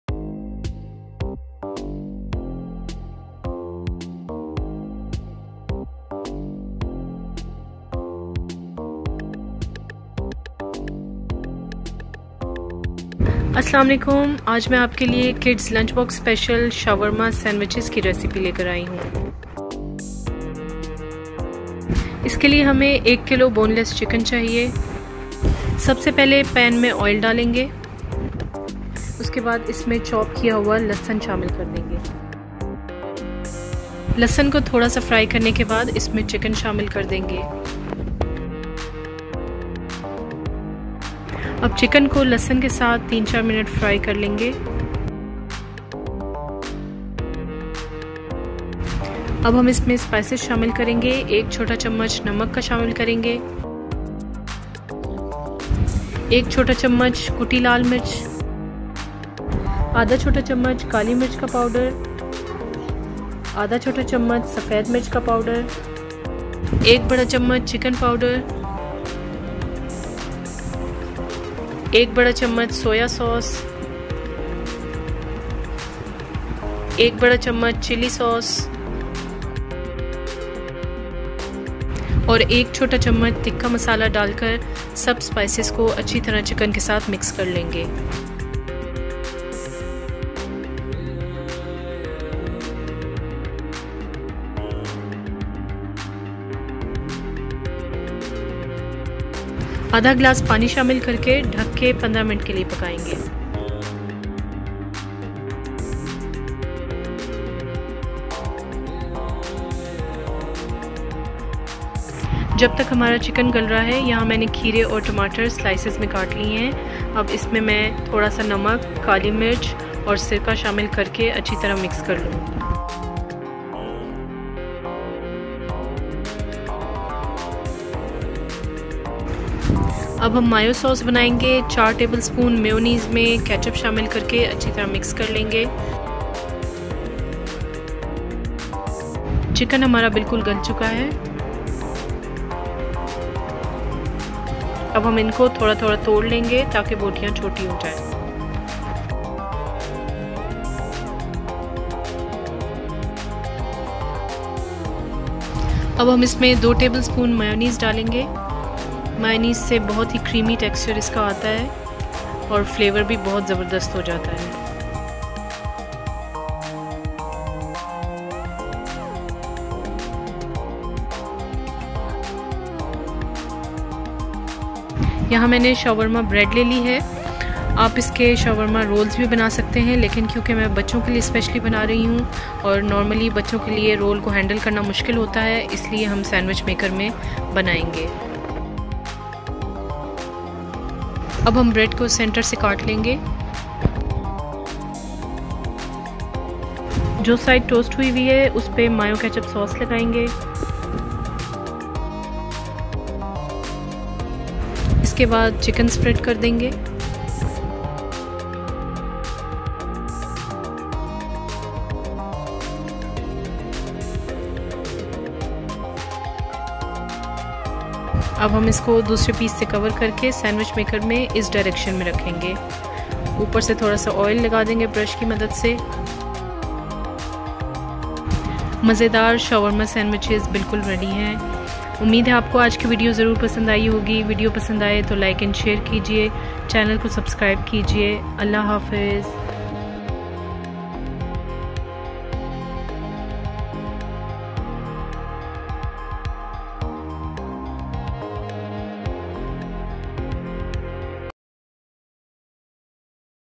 Juicy Shawarma Sandwiches in Sandwich Maker | Kids Lunchbox Special
Easy to make Shawarma Sandwishes for kids Lunch Box. Juicy & Creamy Flavoured Sandwiches.